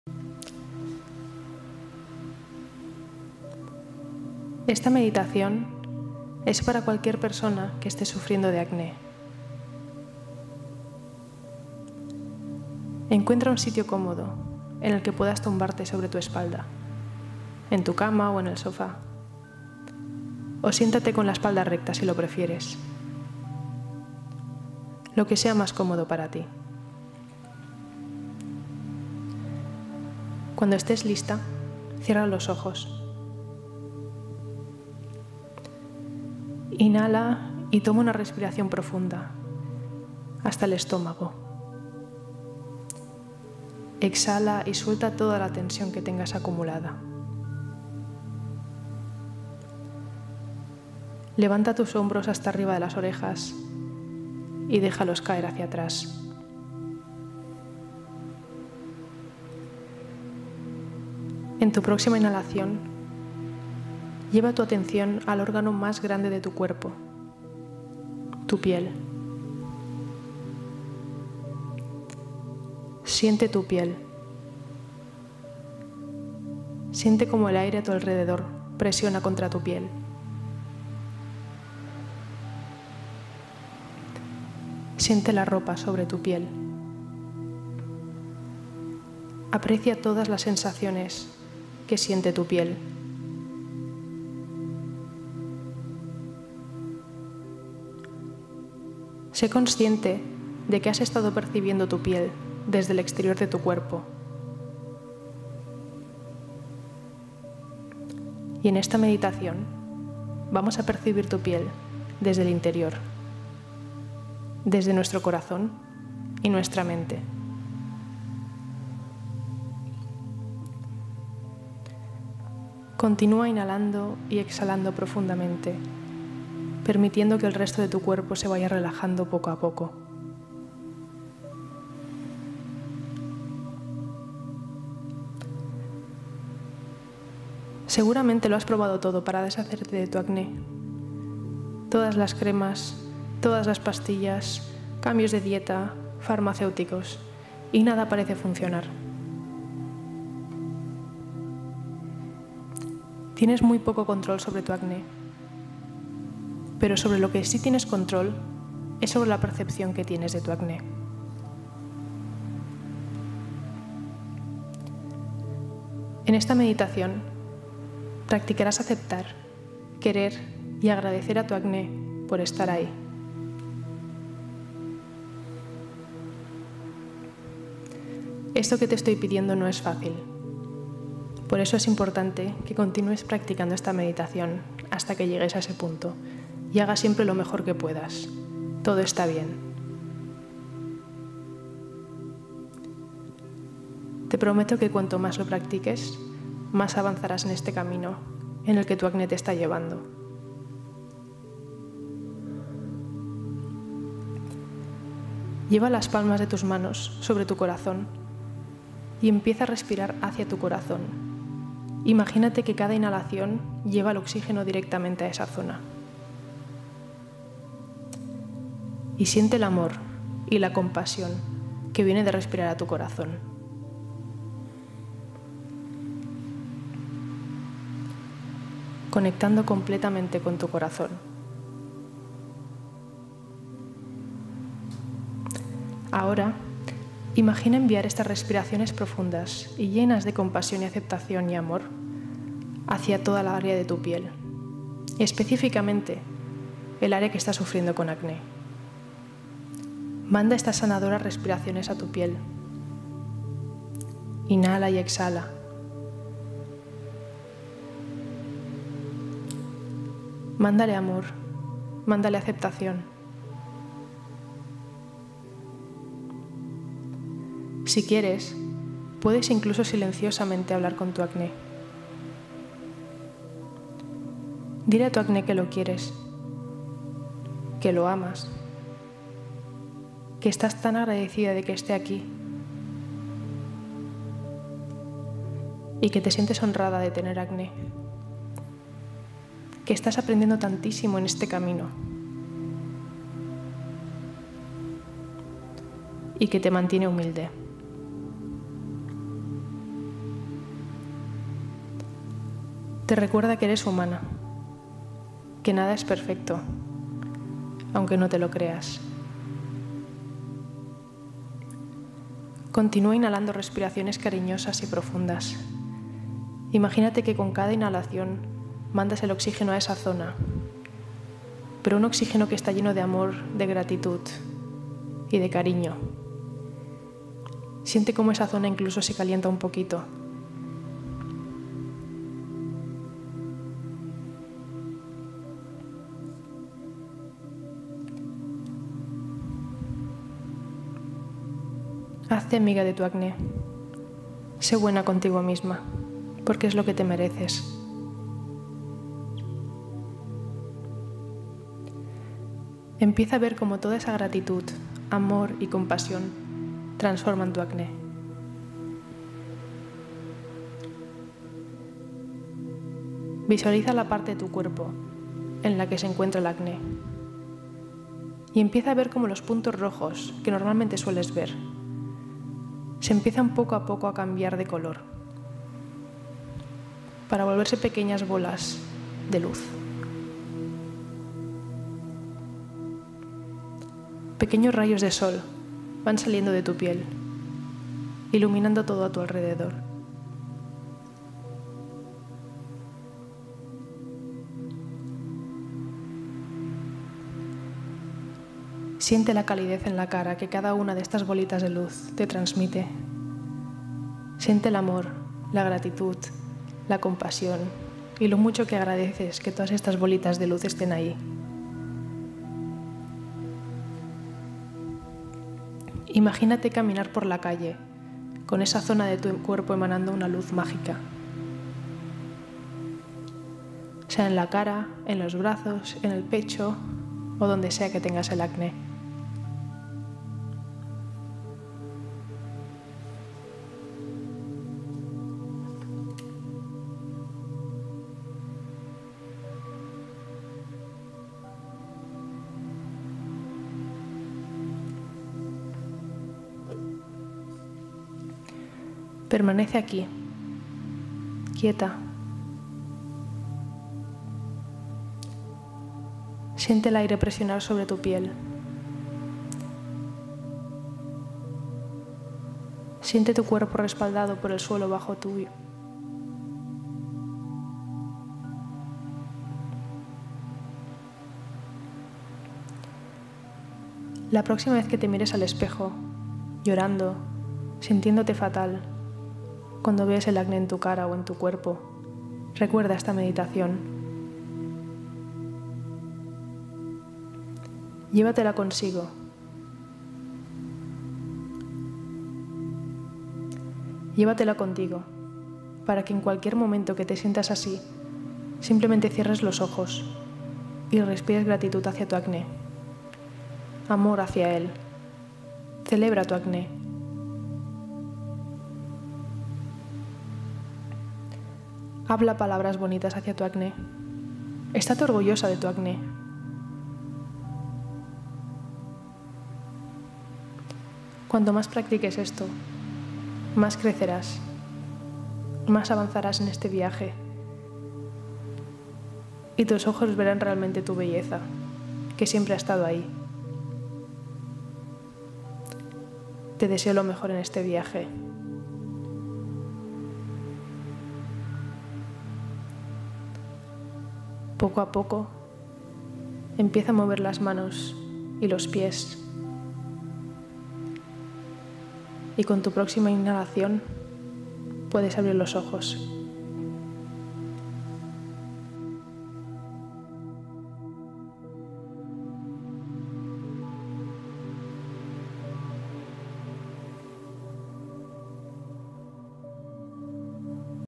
meditacion 21 días para SANAR EL ACNE ✨💆🏻‍♀️❤️🌱 piel más limpia y bonita
Esta meditación de 21 días te ayudará a sanar tu relación con el acné para quererte, aceptarte y tener la compasión que mereces. Puedes hacerla a cualquier hora del día, cuando más te apetezca y puedes seguir haciéndola pasados los 21 días si sientes que necesitas profundizar más en curar el acné :)

Durante el video te guiaré con un poderoso ejercicio de respiración y enfoque mental para ayudarte a relajarte y alcanzar una piel clara y libre de estrés.

Recomendaciones Amazon